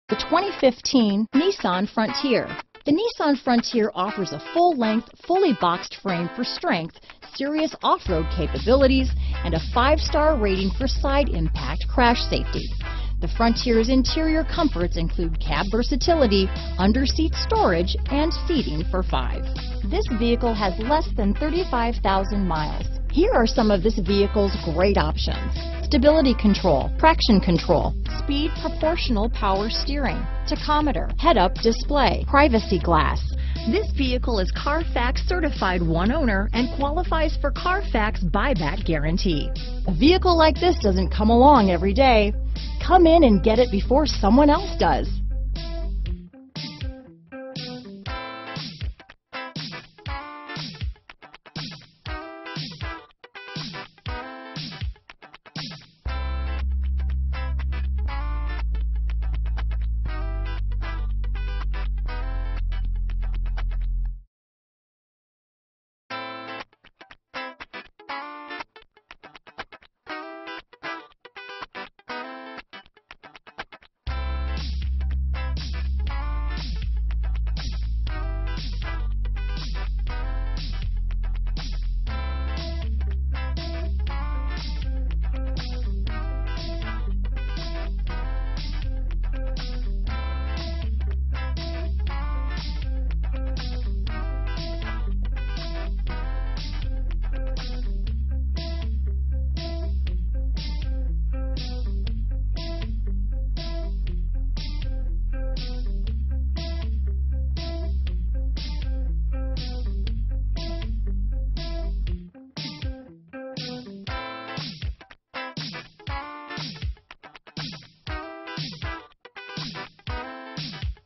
2015 Nissan Frontier Fall River, Dartmouth, New Bedford, Wareham, MA, Tiverton, RI 14211
2015 Nissan Frontier
Prestige Auto Mart: Servicing Fall River, Massachusetts near Dartmouth, New Bedford, Wareham, MA, and Tiverton, Rhode Island.
2015 Nissan Frontier S - Stock#: 14211 - VIN#: 1N6BD0CT6FN748442

Great MPG: 23 MPG Hwy** CARFAX 1 owner and buyback guarantee* PRICES SLASHED!!! OUR LOSS IS YOUR GAIN*** Safety equipment includes: ABS, Traction control, Curtain airbags, Passenger Airbag, Stability control...Other features include: Manual Transmission, Air conditioning, 152 hp horsepower, 2.5 liter inline 4 cylinder DOHC engine, 4 Doors...